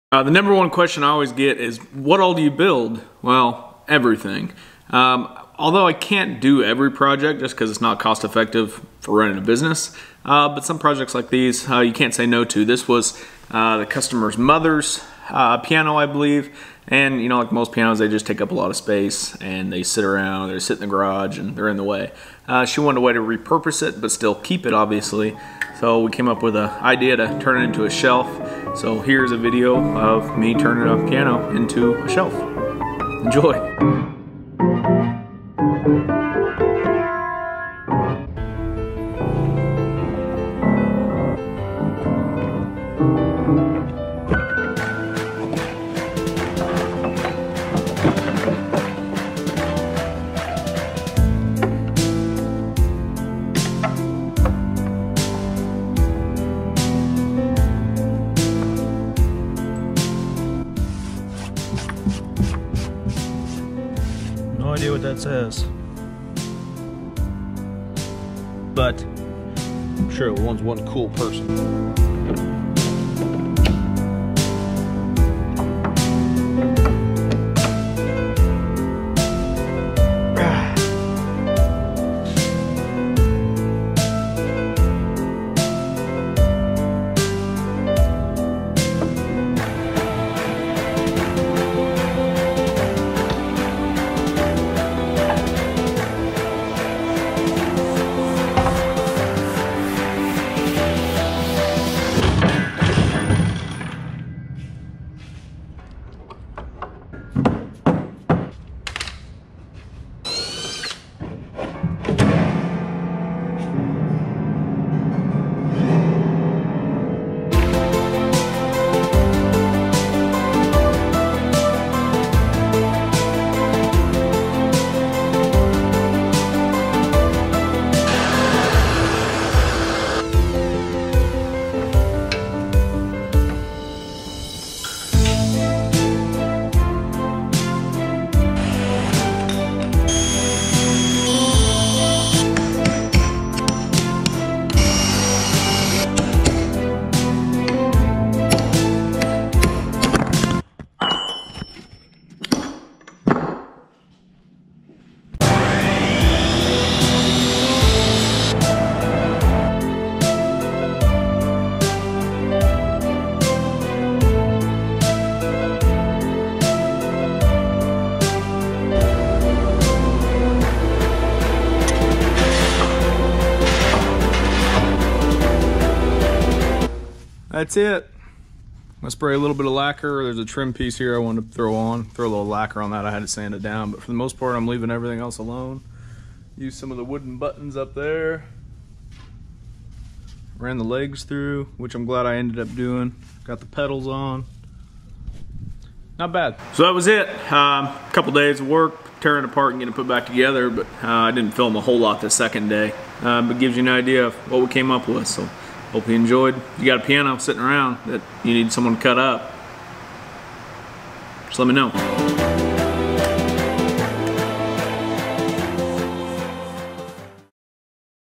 building a piano shelf
turning a piano into a shelf!